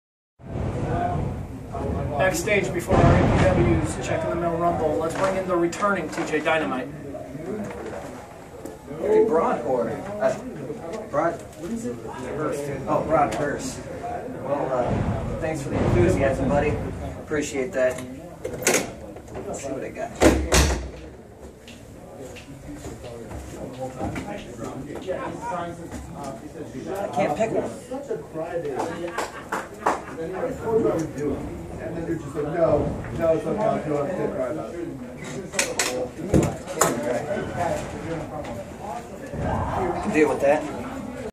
TJ Dynamite picks a number!
The Returning TJ Dynamite selects a number for The Check in the Mail Rumble. In this backstage exclusive hosted by Harry Broadhurst & Richard Jeremy.